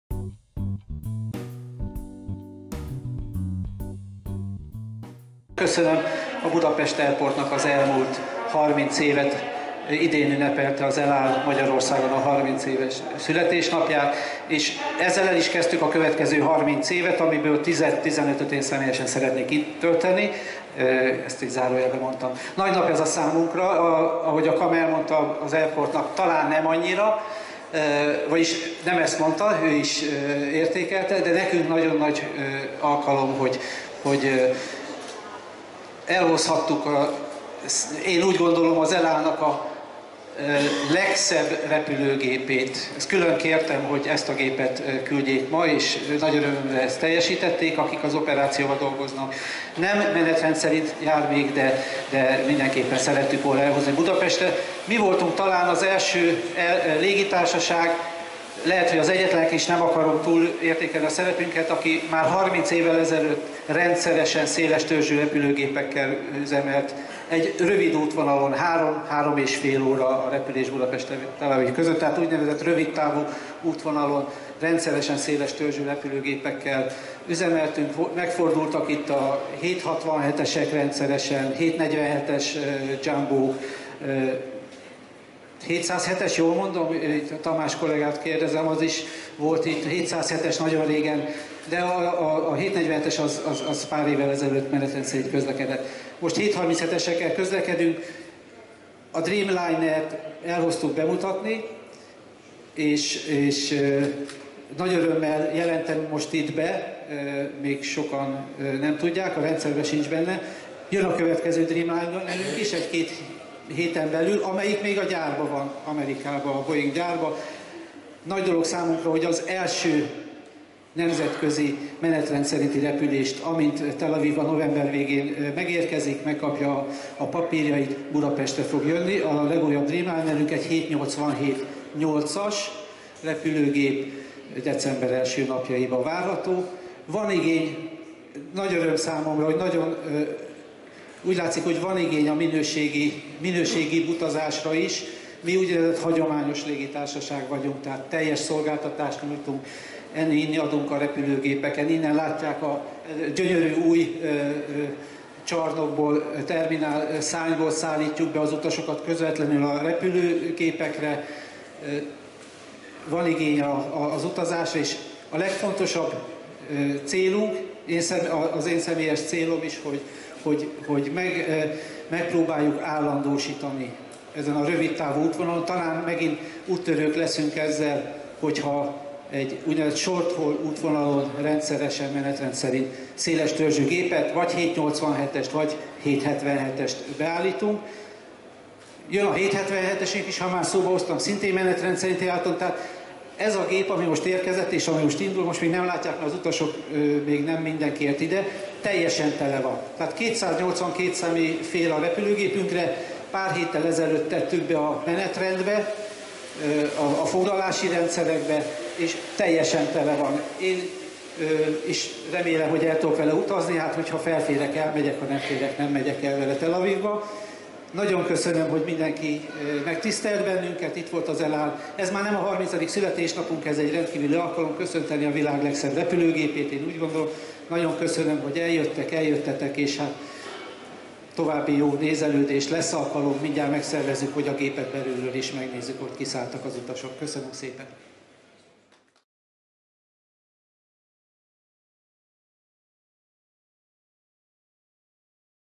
Váradi János: Rendszeresen repülne szélestörzsű géppel Budapest és Tel Aviv között az EL AL
Váradi János, az EL AL Israel Airlines magyarországi igazgatójának beszéde vágatlanul a légitársaság első, menetrendszerűen Budapestre érkező Boeing 787-9 Dreamliner típusú, szélestörzsű utasszállító repülőgépének érkezése alkalmából.

Liszt Ferenc Nemzetközi Repülőtér (BUD/LHBP)
Budapest, 2019. november 18. (hétfő)